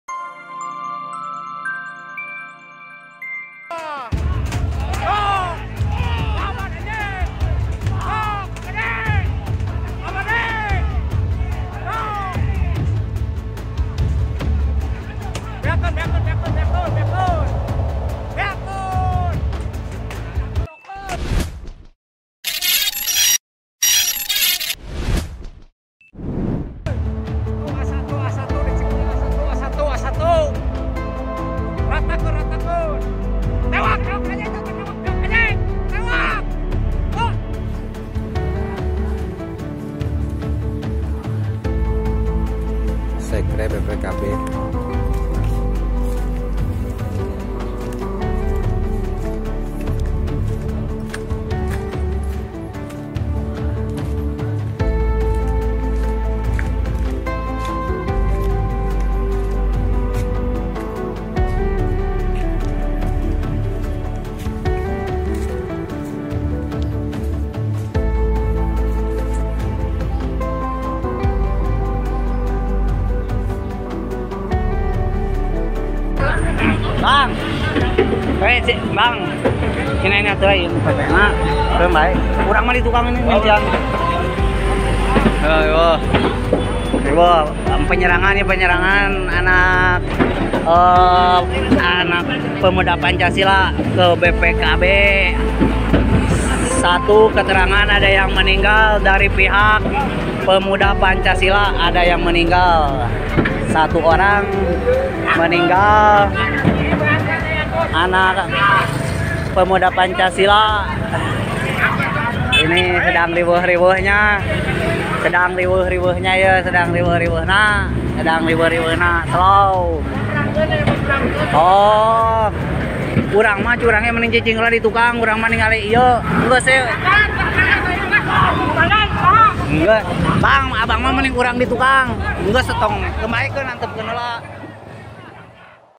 Bentrok Maut Dua Ormas di Perbatasan Sukabumi-Cianjur
Bentrok ormas Pemuda Pancasila (PP) dan Badan Pembinaan Potensi Keluarga Besar banten (BPPKB) Banten terjadi di perbatasan Cianjur-Sukabumi Minggu (26/9), pukul 15.00 WIB.

Polisi sudah mengamankan sejumlah barang bukti, berupa senjata tajam hingga sepeda motor anggota ormas yang terlibat bentrok.

Kapolres Sumedang AKBP Eko Prasetyo Robbyanto mengatakan bentrokan yang melibatkan kedua ormas tersebut dipicu
oleh kesalahpahaman.

Kejadian itu bermula saat salah satu ormas diduga mengganggu proyek yang sedang diamankan ormas lainnya.